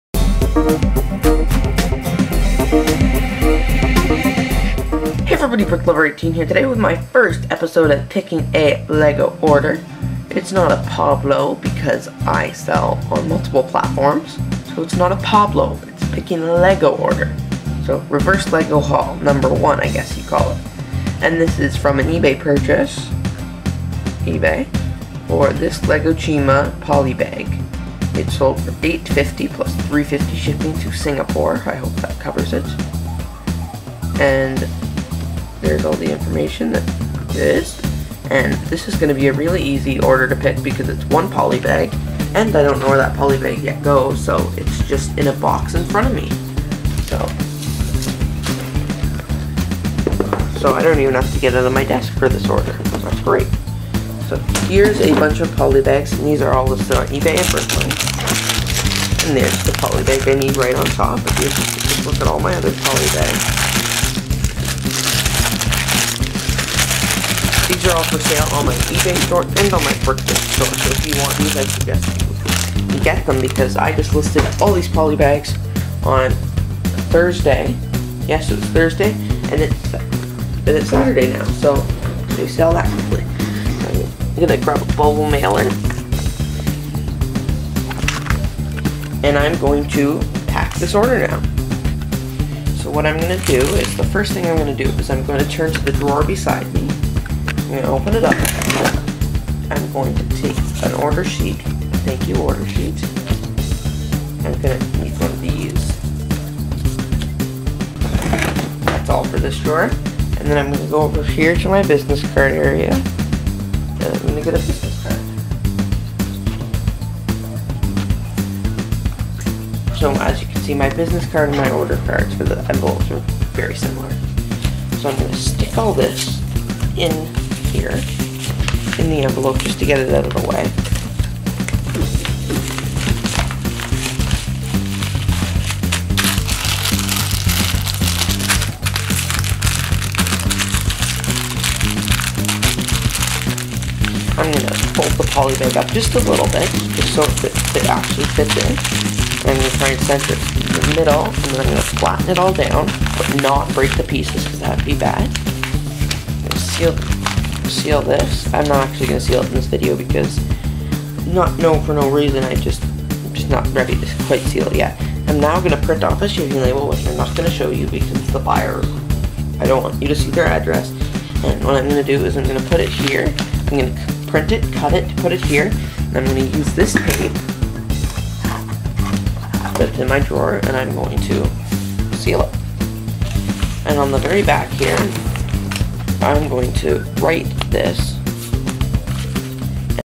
Picking And Packing A LEGO Order! "PAPALO" #1 - eBay!
In this video you see me "P"icking "A"nd "P"acking "A" "L"EGO "O"rder! This order is for eBay!

If you want a PAPALO please let me know in the order, also tell me your username/ that you are on YouTube for a shoutout! I am going to try and do a PAPALO for every order!

Today's Video Question: Do you like this new series PAPALO?
*please anwser in the comments!

A: Yes